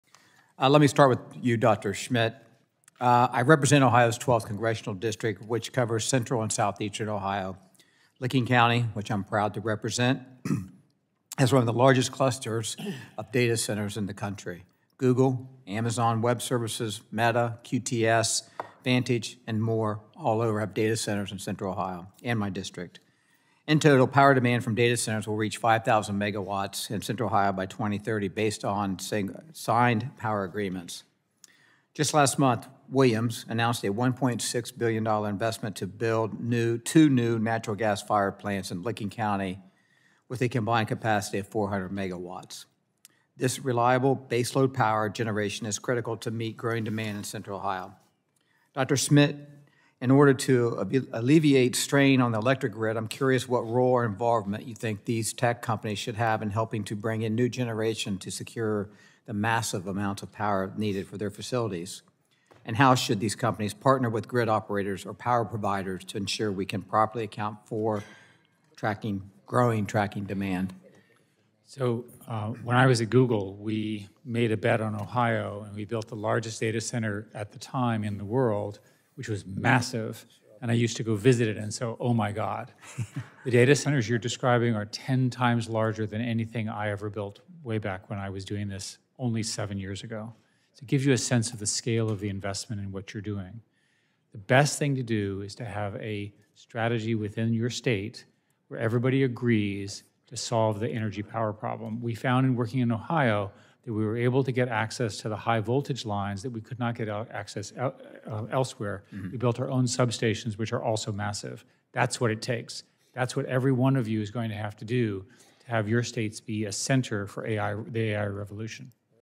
Balderson Discusses Ohio Data Centers and the Need to Meet Growing Power Demand
At an Energy and Commerce Committee hearing, Congressman Troy Balderson (OH-12) questions tech expert Dr. Eric Schmidt, chair of the Special Studies Project, on tech companies’ role in strengthening the U.S. electric grid amid AI-driven data center boom.